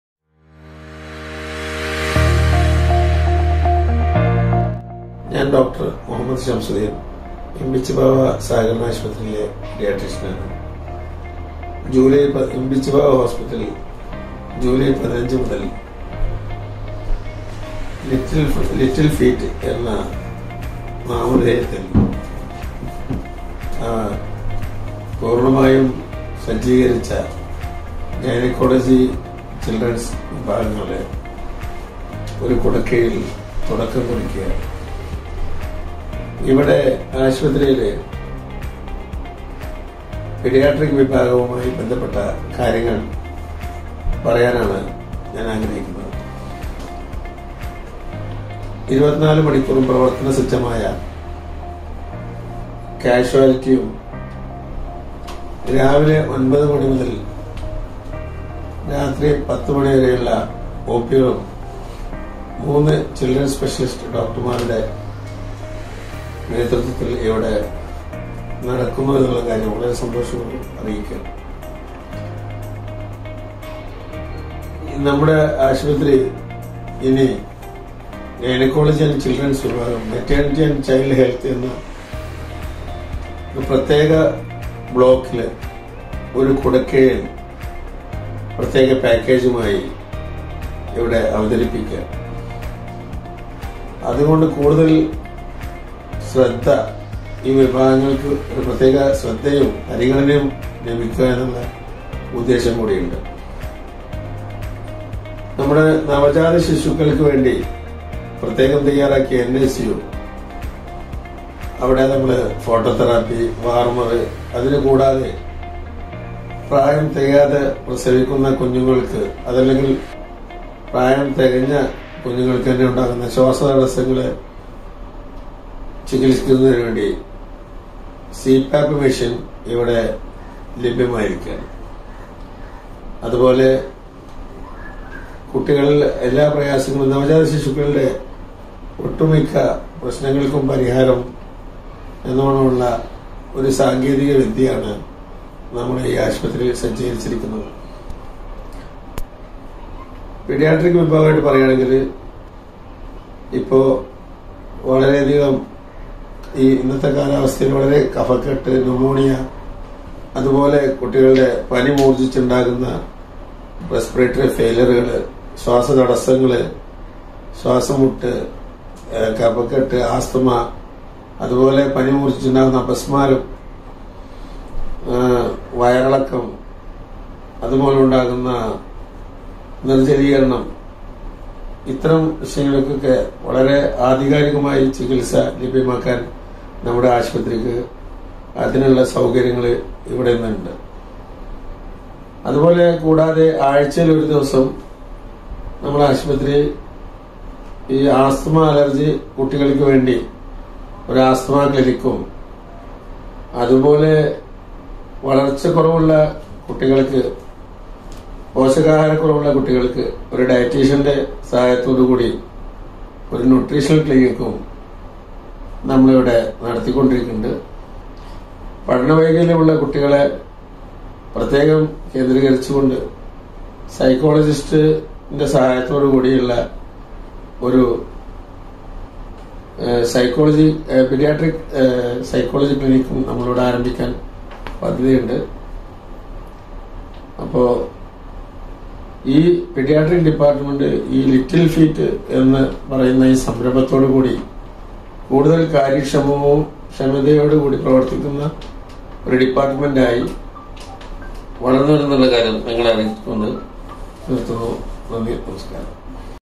IMCH LITTLE FEET
വിപുലീകരിച്ച
ഗൈനക്കോളജി
പീഡിയാട്രിക്
ഫെർട്ടിലിറ്റി വിഭാഗത്തെ കുറിച്ച്
പ്രശസ്ത ശിശുരോഗ വിദഗ്ദ്ധൻ
ഡോ. എൻ.മുഹമ്മദ് ഷംസുദ്ധീൻ സംസാരിക്കുന്നു